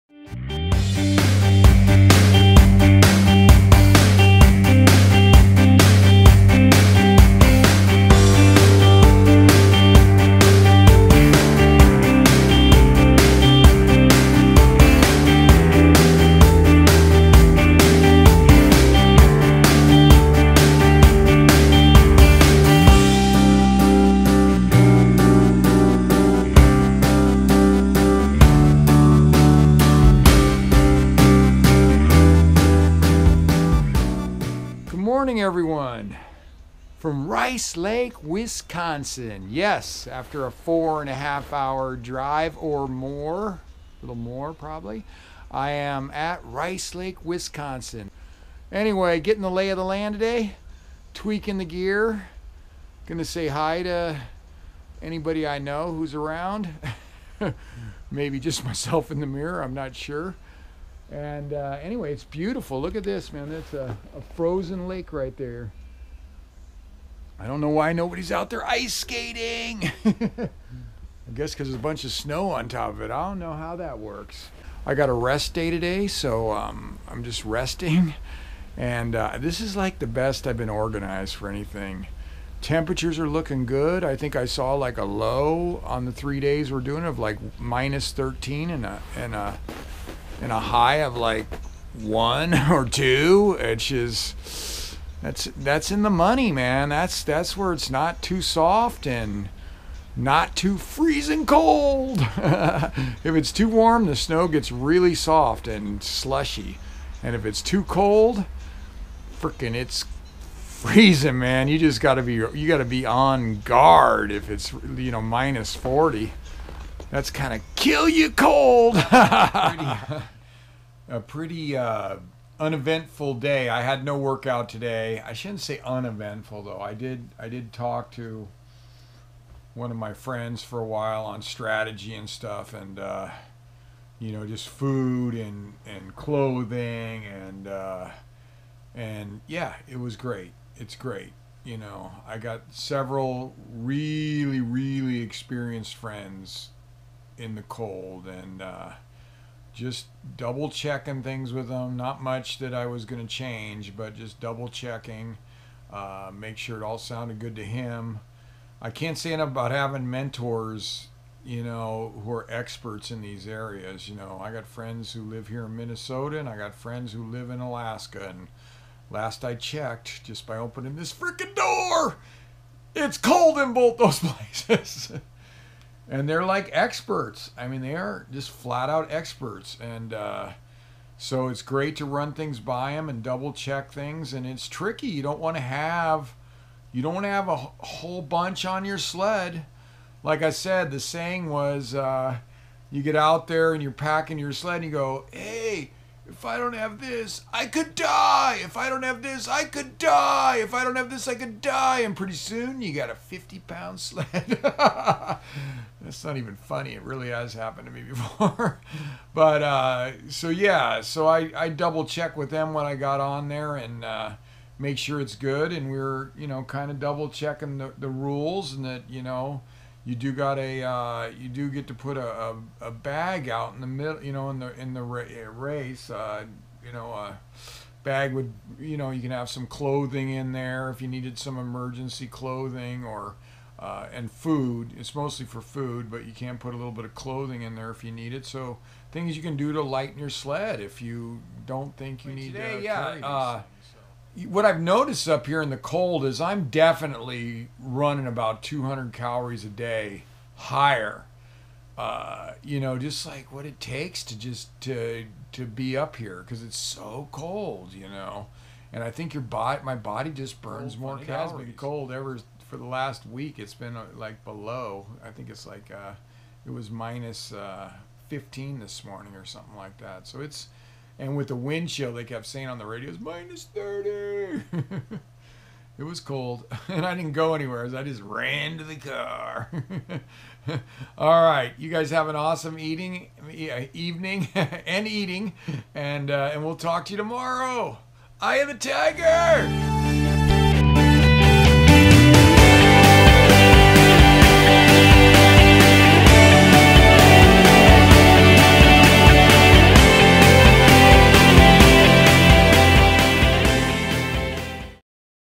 VLOG 11 INTERNATIONAL FALLS MN
This is the 11th Vlog from Epic Bill Bradley as he prepares for the Arrowhead 135.   Bill is at Rice Lake, Wisconsin and has a day off.  His body is getting acclimated to the harsh winter weather.  Temps and snow have been dropping continuously.  This will be Bill’s 7th attempt on Jan.29, 2018 – 31st. It’s a 135-mile foot race on frozen snow-covered tundra, International Falls MN boasts record low temps in the -50 degree range.

This comes from the mind of a man who defines “Mental Toughness”!  Bill proved this when he became the fourth person ever to complete a Quad Badwater totaling 584 miles on foot in Death Valley.  He also still holds the world record for 7 continuous rim to rim runs across the Grand Canyon!

We hope you enjoy these truly inspirational vlogs designed to give you an honest view of an Extreme Endurance Athlete.